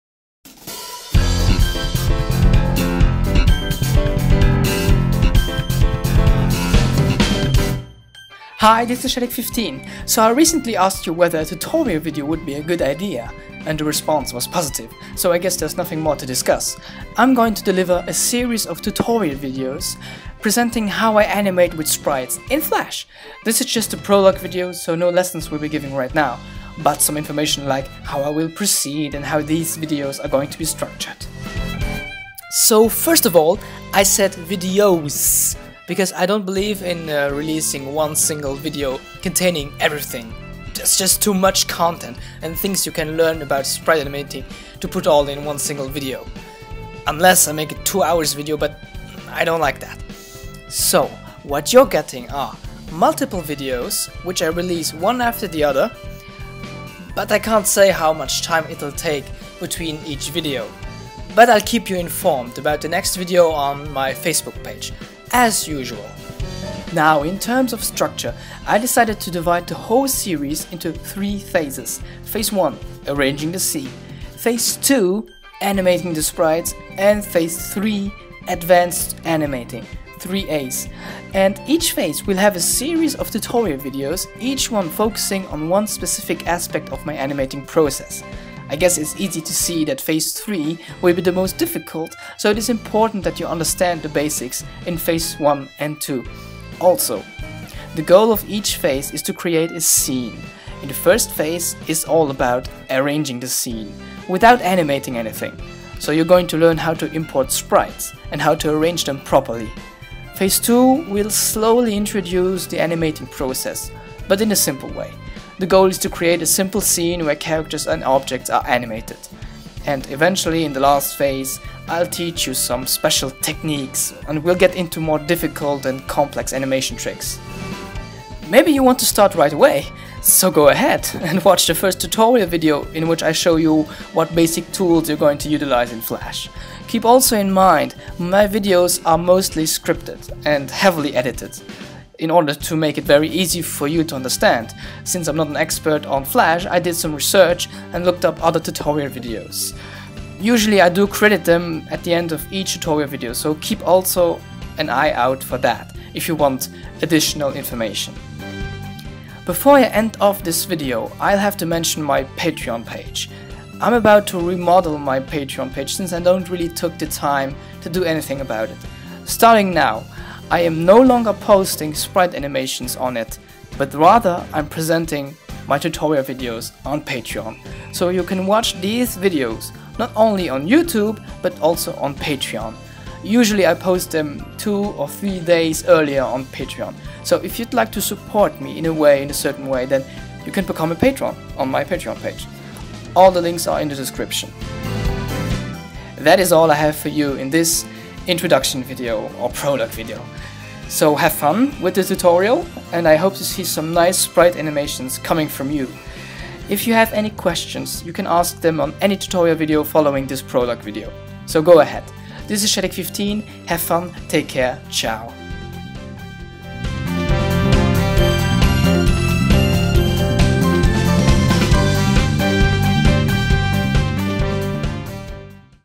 Prolog Video :: Tutorials :: [How to Sprite Animate]
Hello, Shadic15 here.

It's time to start the Tutorial series I was talking about for a while. So, here we are, this is the introduction video (I so "appropriately" called it prolog video) which explains how this series will play out. Enjoy :)